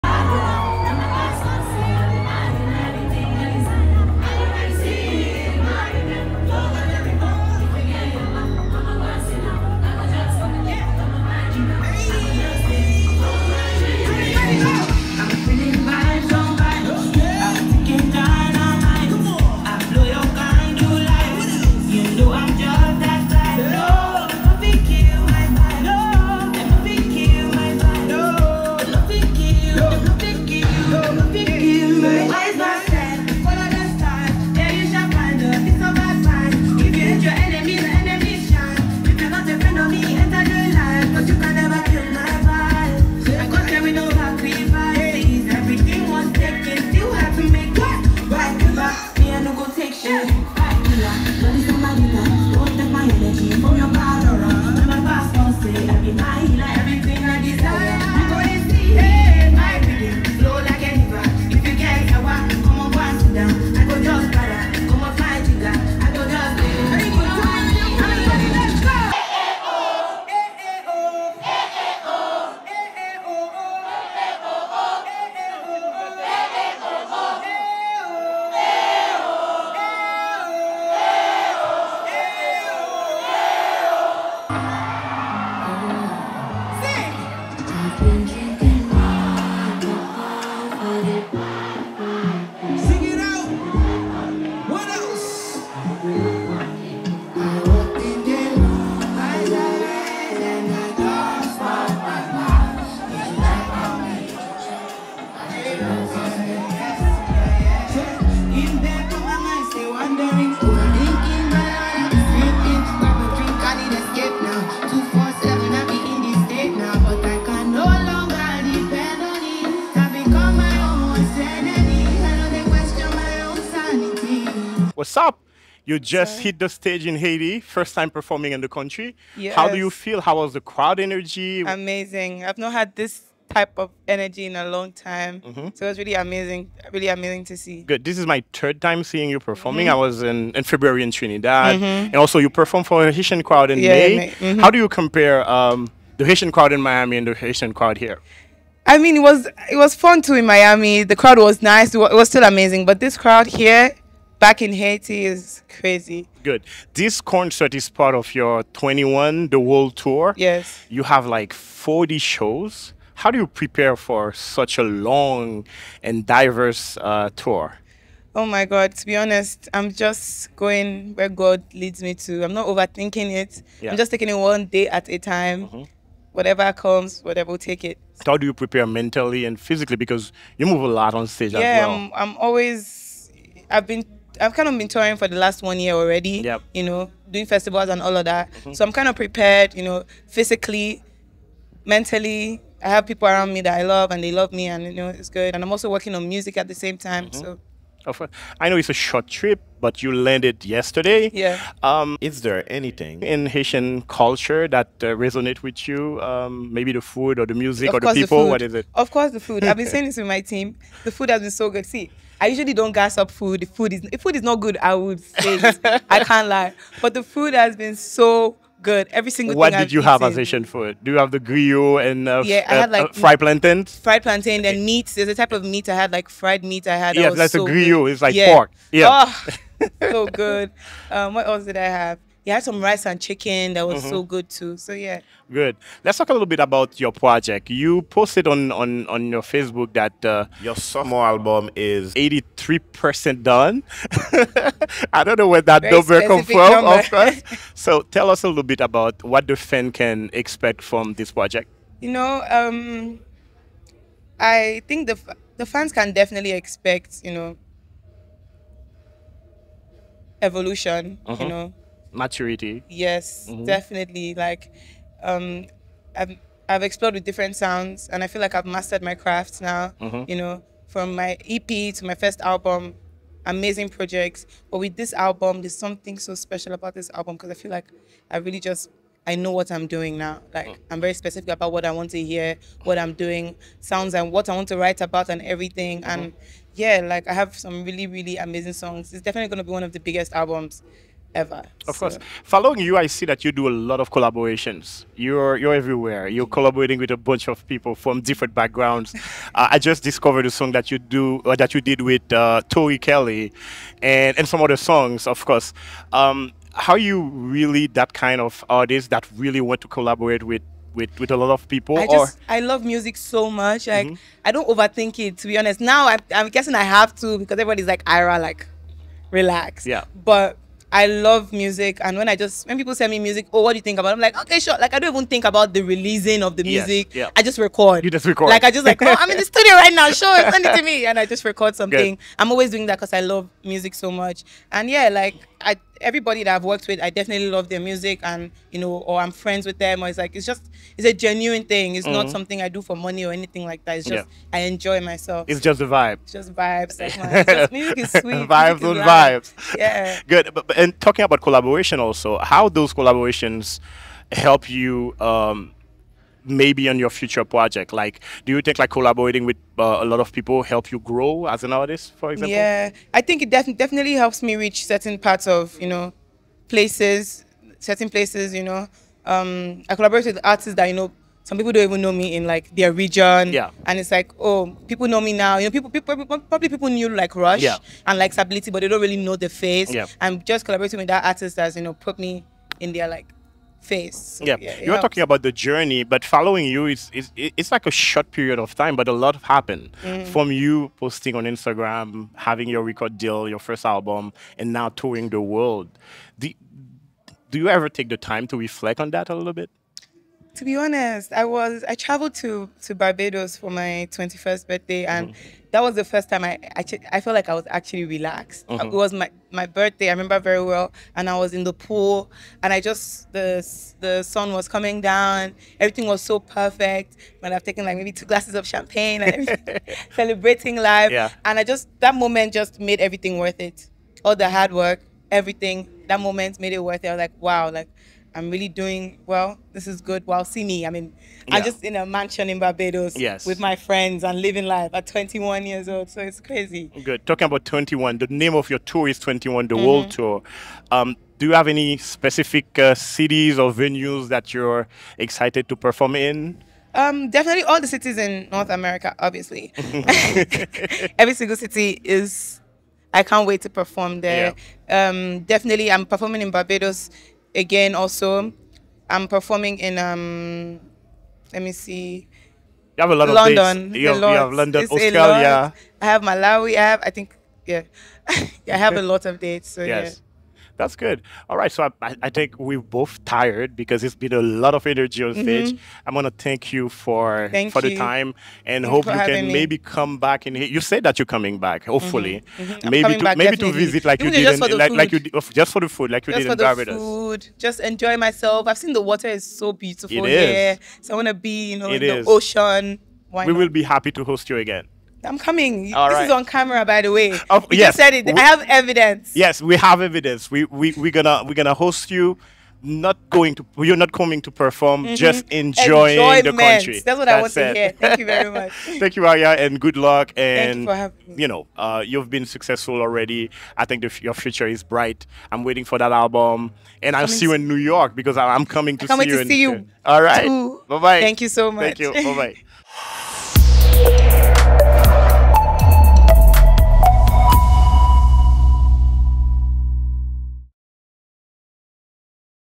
Ayra Starr Exclusive Interview in Haiti : World Tour, Love for Haitian Food & Album Teasers
Join us in this exclusive interview as Afrobeat sensation Ayra Starr reflects on her thrilling first performance in Haiti, shares her newfound love for Haitian food, and offers intriguing teasers for her upcoming sophomore album. Ayra provides insights into her journey, collaborations, and future plans, from her humble beginnings to her evolution into a world-tour headliner.

00:00 Performance Recap
02:06  Performing in Haiti for the First Time
02:36 Preparing for the World Tour
03:18 Falling in Love with Haitian Food
03:56 Sophomore Album Teaser: A More Mature Sound
04:52  Collaborations
06:27  From Covers to Stardom
07:21 Specific Cities & Venues She's Excited About